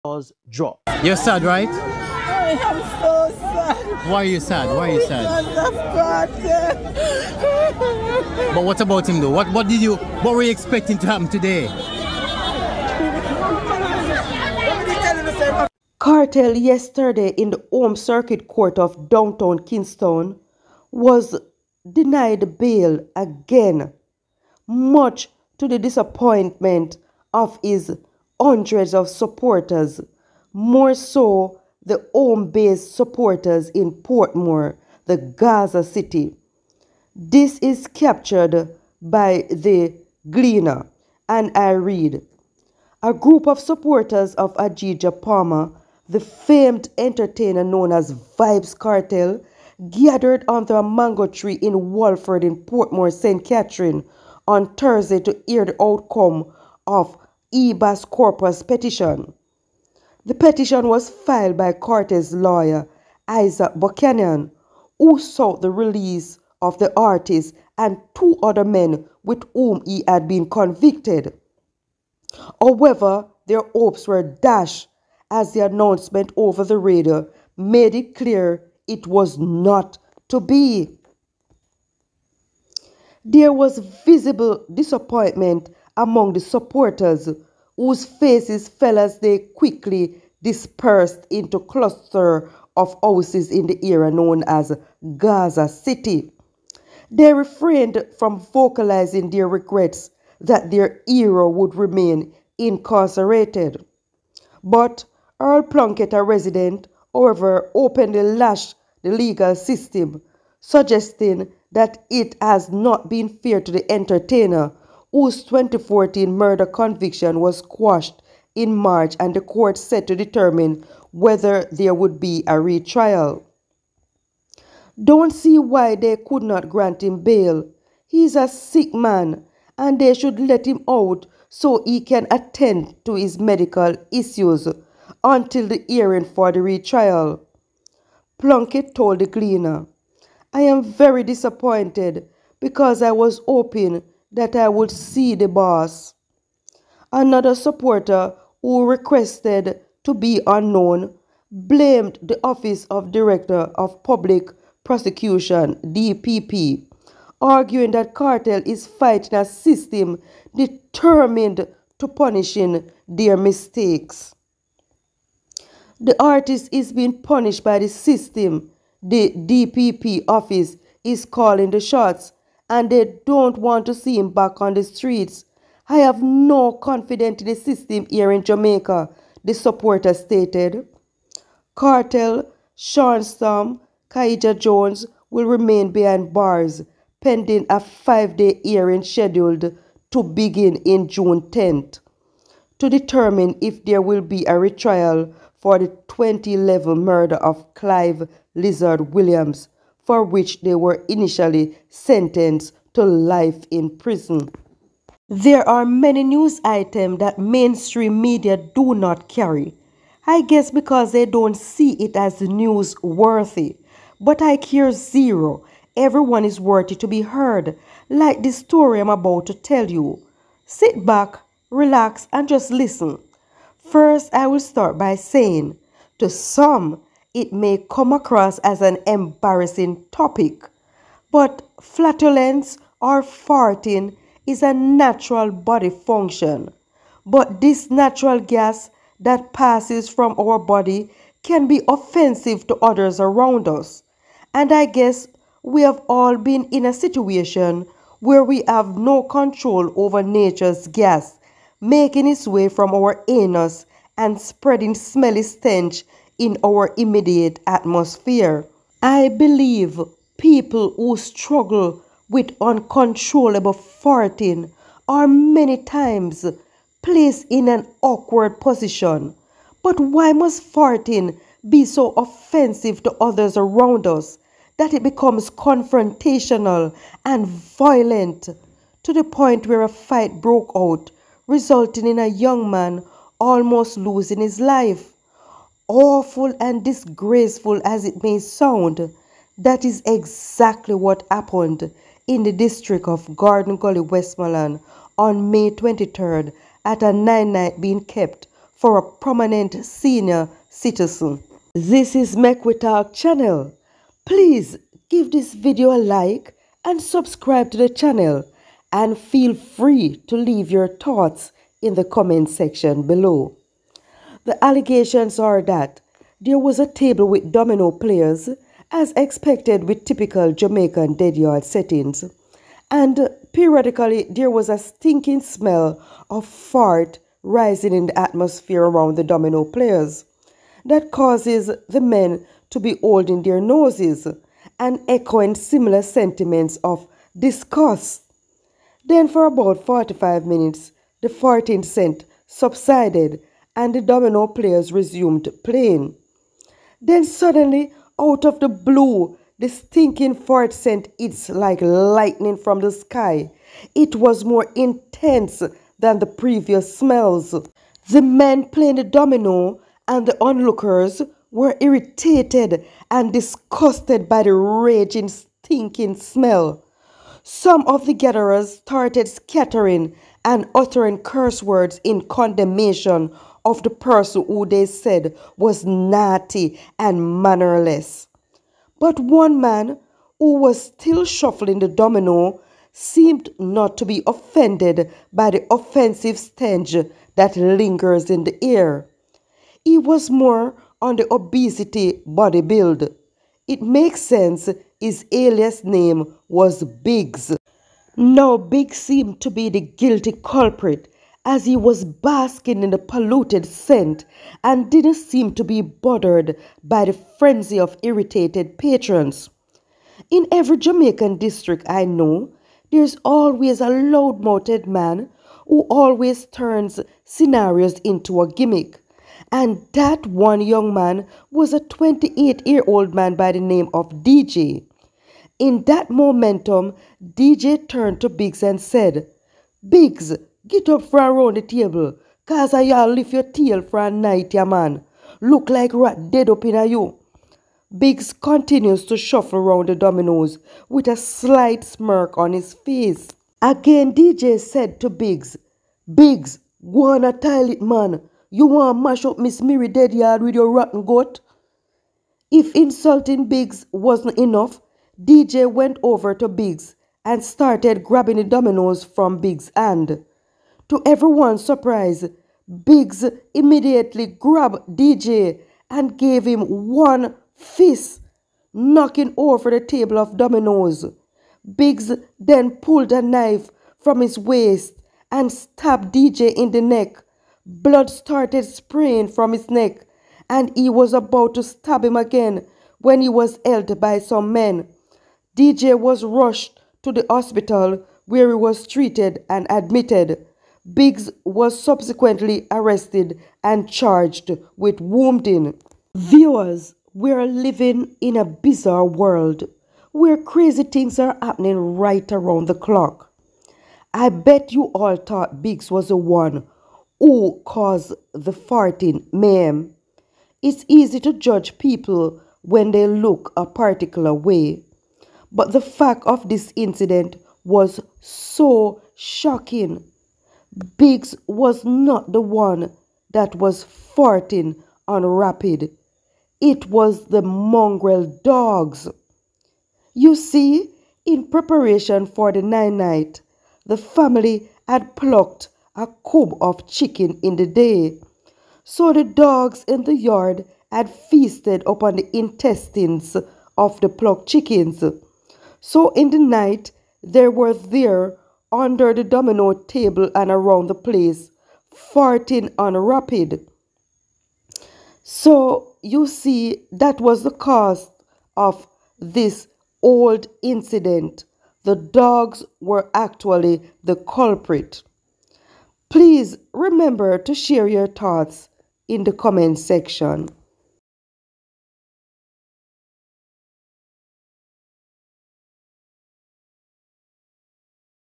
Kartel fan ketch heartattack + man st@b up man ova St!nking F@rt‼️😱
Vybes Kartel was yesterday denied bail in the circuit court of downtown Kingston Jamaica, leaving his fans heart broken.  Man got stabbed in Westmoreland for accusing overweight man of farting stink.